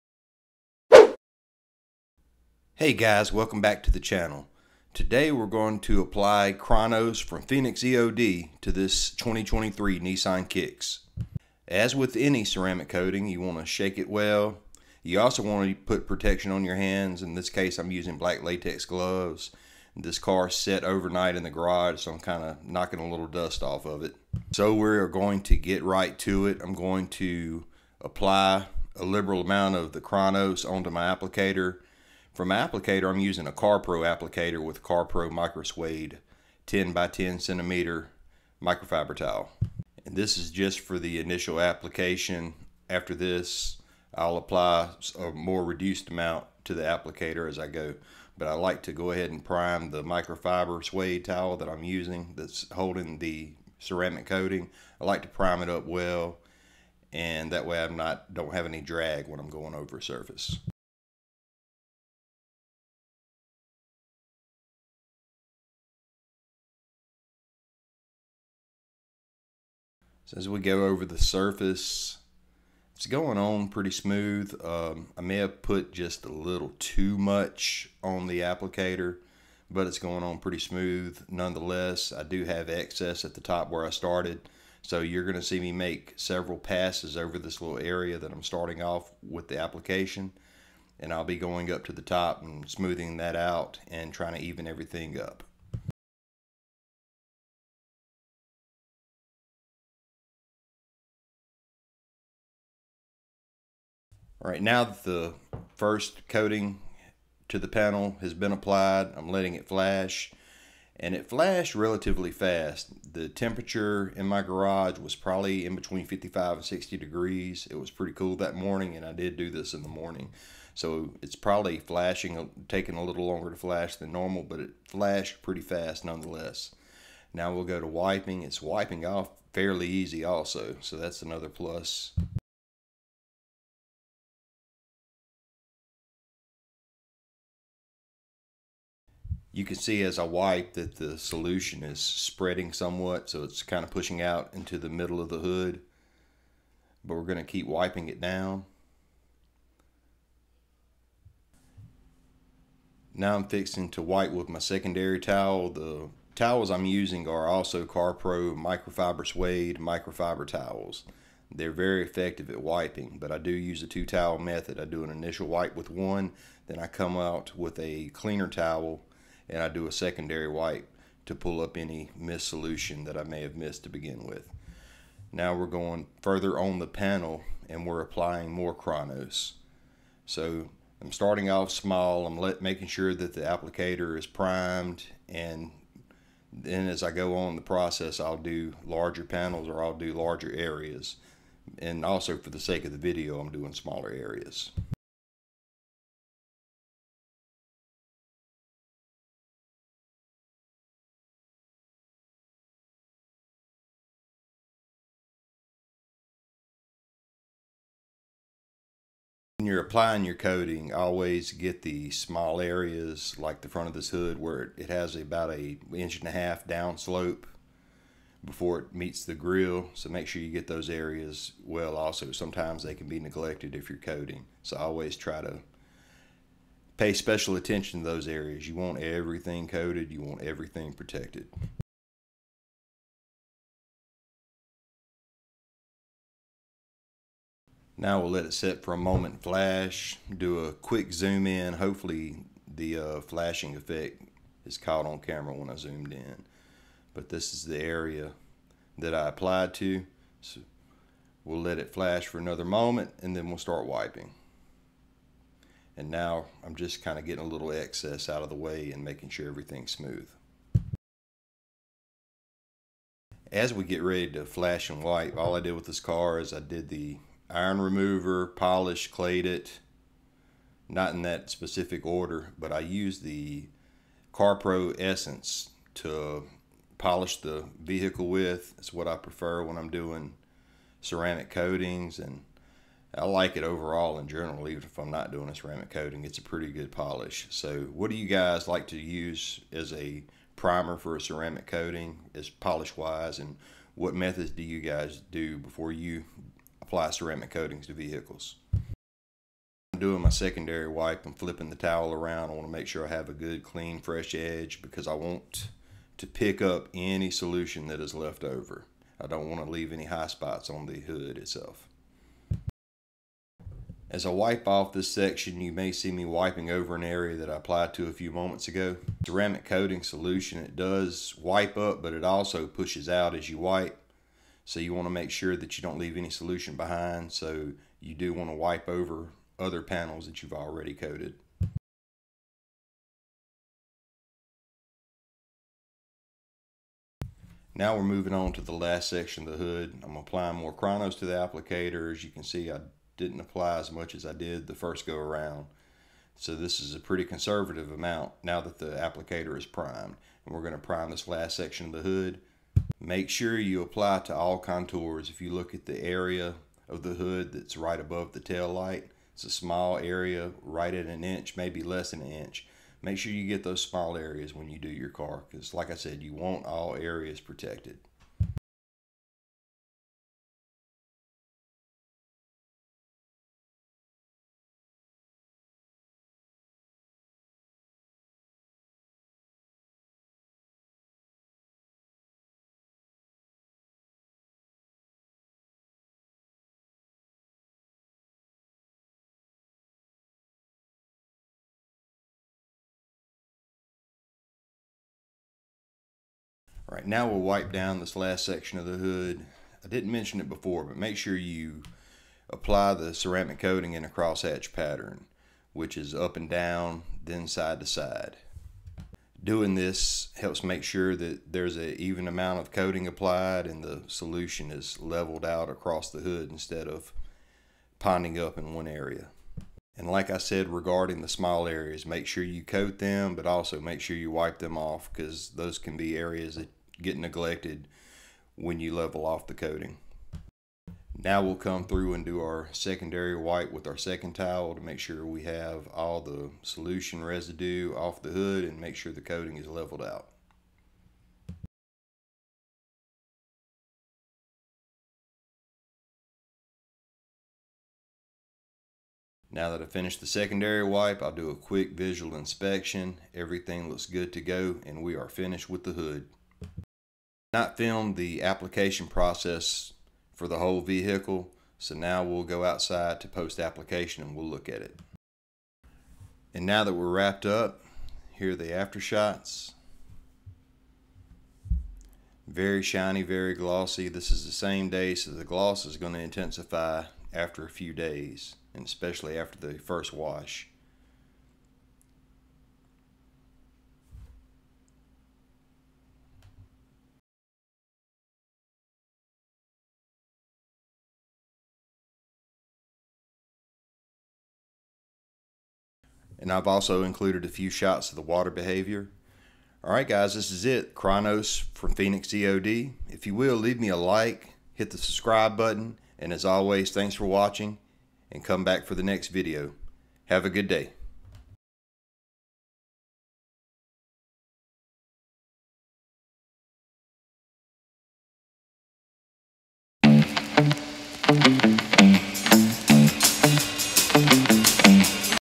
Will Phoenix EOD Kronos Help Paint Defy Time? Let's Apply and Find Out
In this Video I apply @Phoenixeod  Kronos to this 2023 Nissan Kicks in Boulder Gray. Watch the application process and see the finished product at the end.
Items Used:
Phoenix EOD Kronos Coating

Music from Uppbeat (free for Creators!)